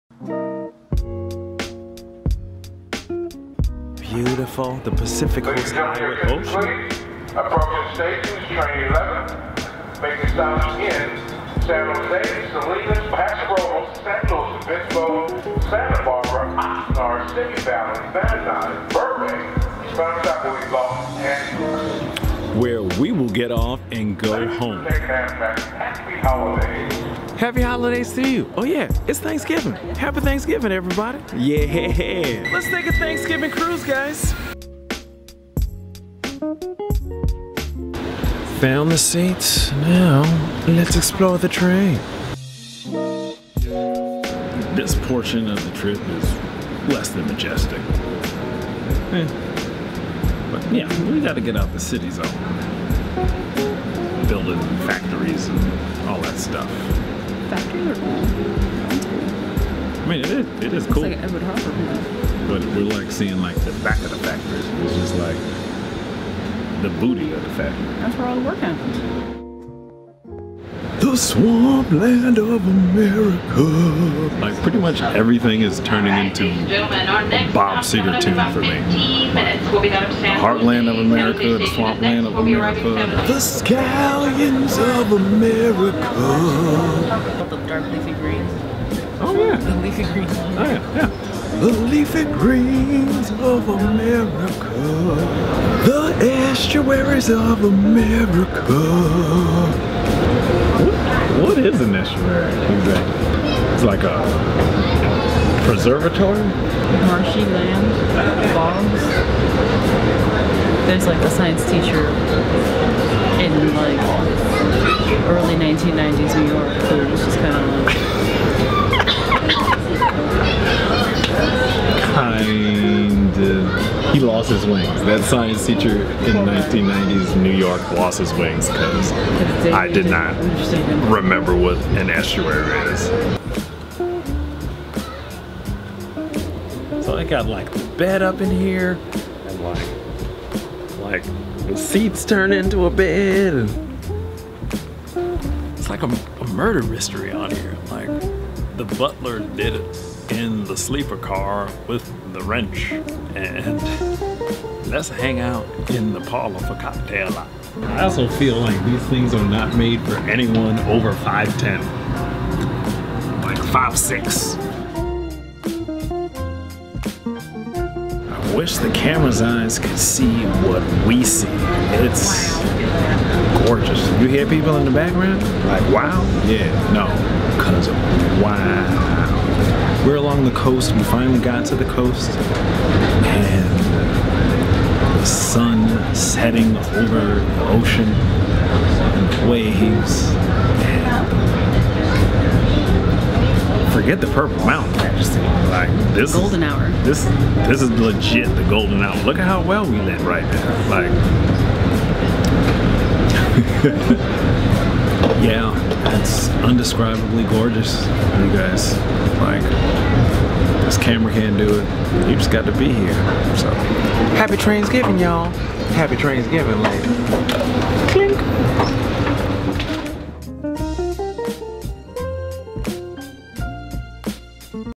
Thanksgiving Coastal Train Ride! 🚄
Turned Thanksgiving into "Trainsgiving" with a day long train ride from Oakland to LA!